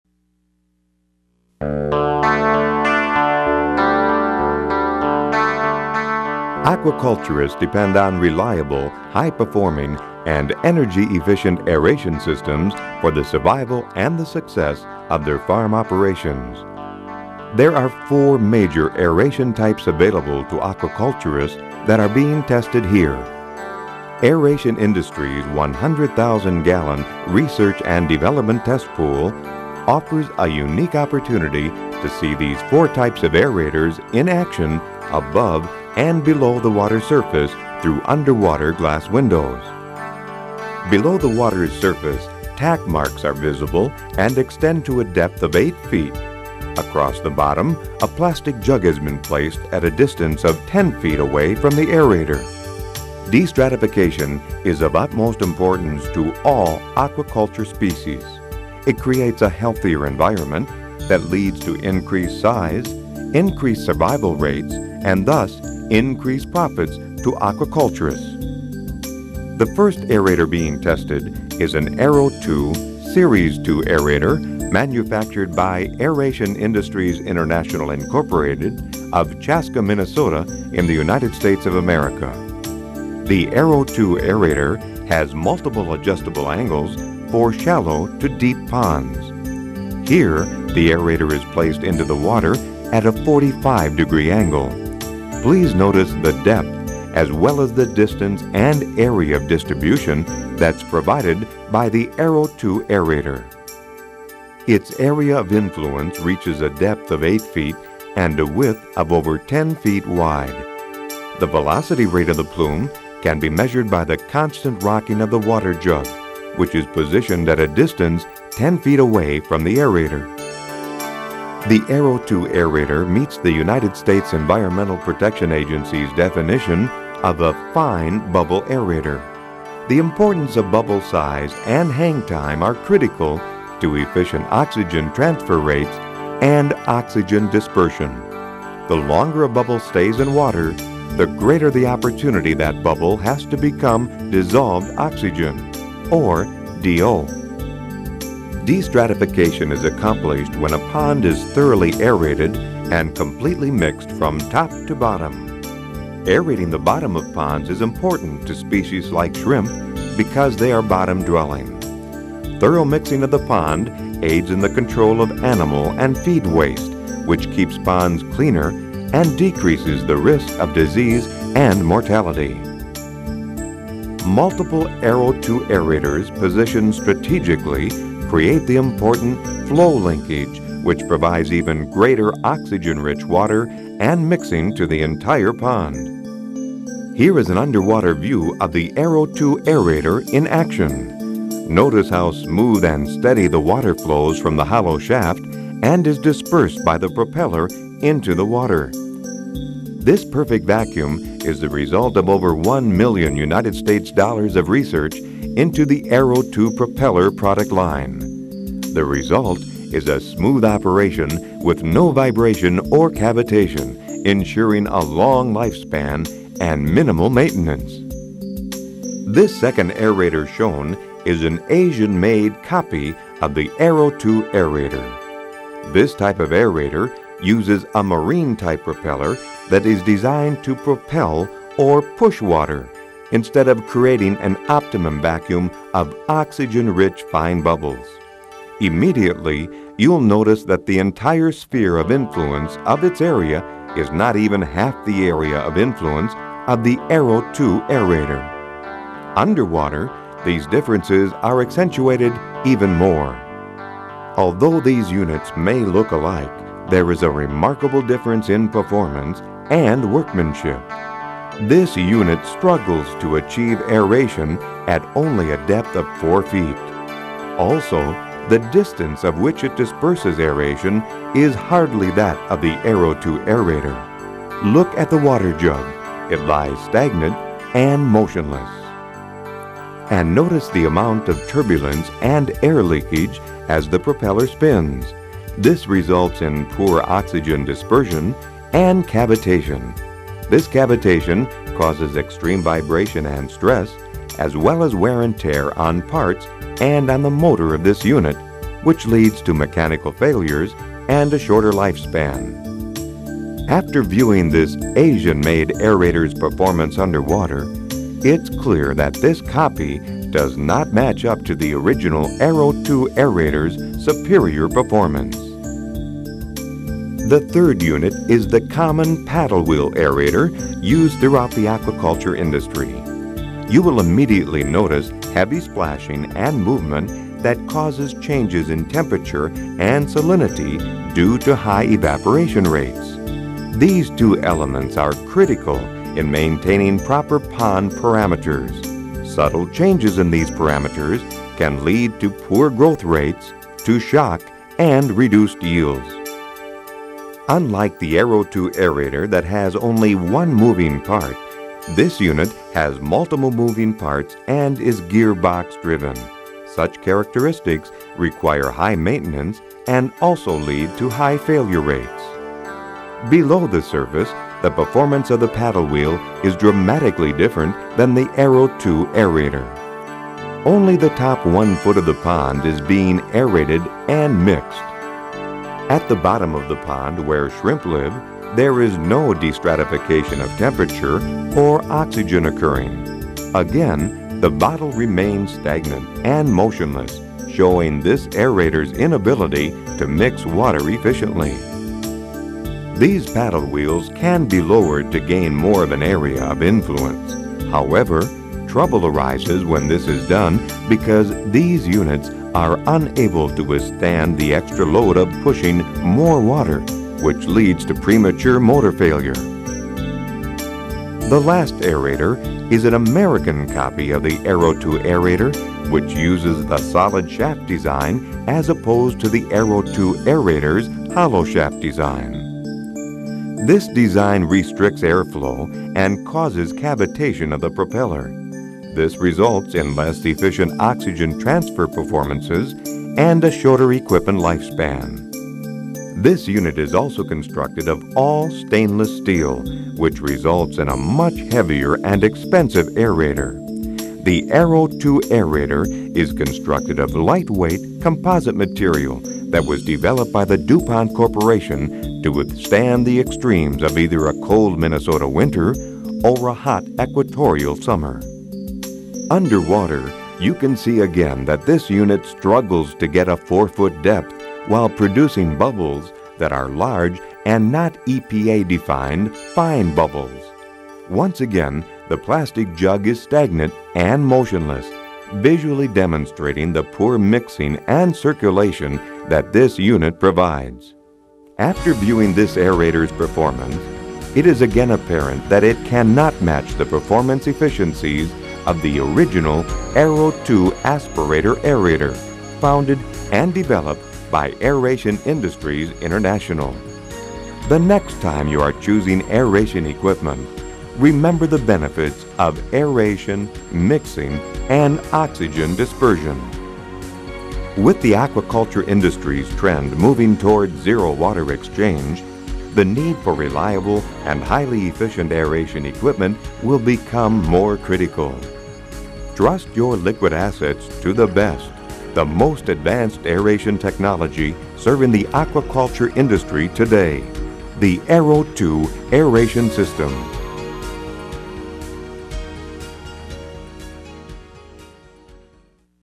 Aeration Industries International Series II Aquaculture Aerator (English)
A detailed look at Aeration Industries International's popular Series II Aquaculture Aerator. for more info.